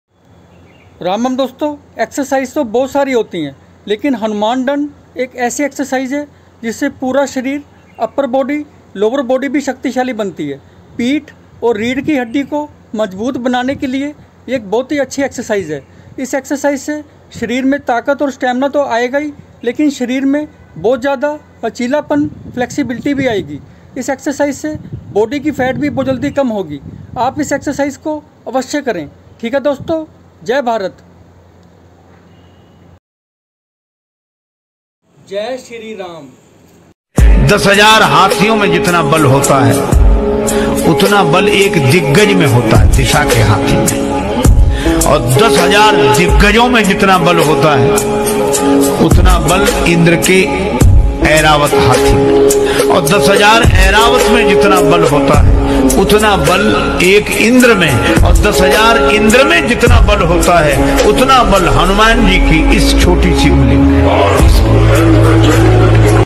Hanuman dand लगाने से पूरा शरीर मजबूत बनता है Full body hiit exercise at home
Jai Bajrangbali🙏Ram Ram Doston❤️Hanuman dand lagane se pura sharir majboot banta hai. Hanuman dand lagane ke bahut sare fayde benefits hote hain. Iss exercise se peeth back spine strong hoti hai. Iss behad shaktishali vyayam se sharir body mein bahut zyada stamina endurance, takat strength power, aur lachilapan flexibility aati hai aur sharir ki charbi body fat bhi kam reduce hoti hai. Aap hanuman dand desi vyayam ko jarur karen. Aap iss high intensity exercise ke 3 to 5 sets kar sakte hain aur one set mein 10 to 15 dand push ups laga sakte hain.
Dhanyawad🙏
Piyush Garg Home Fitness 🇮🇳
Jai Bharat 🇮🇳